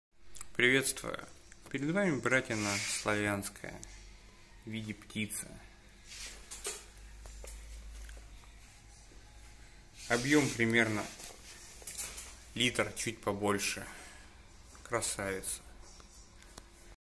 Славянская братина
Братина выполнена из кедра. Ручная работа. Братина птица.